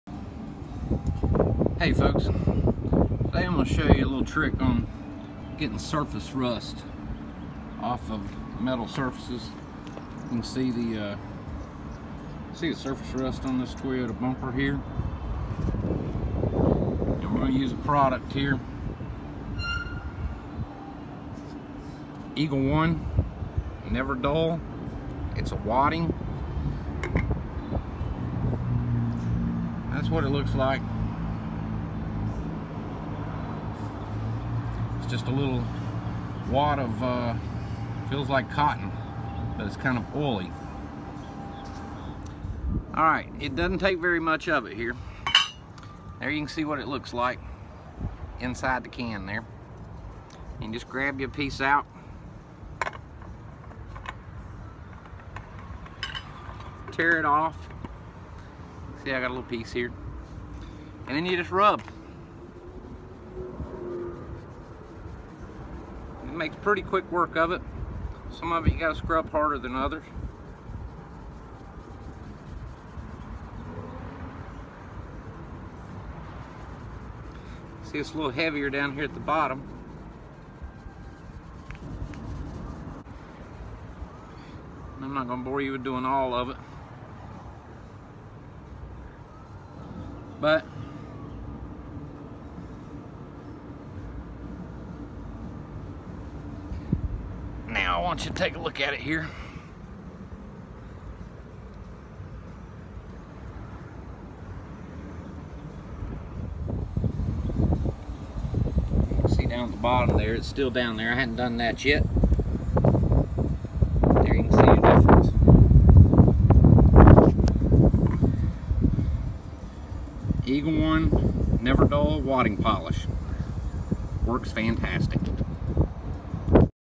Remove surface rust from chrome bumper or wheels Eagle One Never Dull Wadding Polish
Removing surface rust from a chrome bumper or wheels